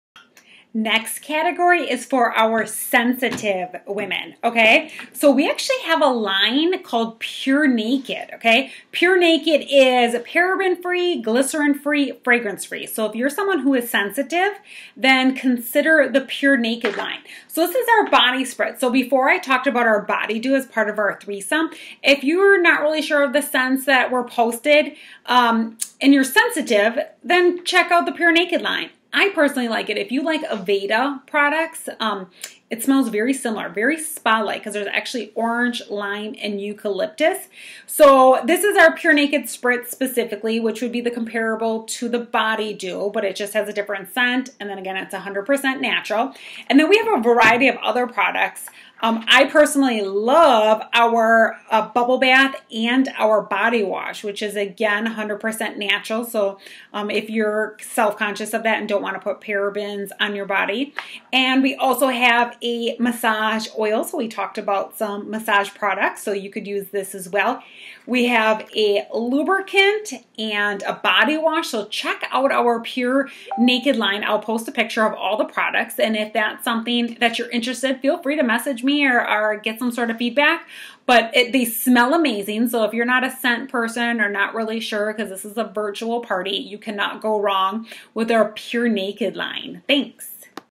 Pure Romance by Tammy Pure Naked Line
Go naked!!
All-natural product line many featuring essential oils and vitamins for refreshed, radiant skin. Line includes body moisturizers, massage oil, lubricant, shaving cream, bubble bath and body wash.

Safe for sensitive skin
All-natural ingredients
Fragrance free
Paraben free
Glycerin free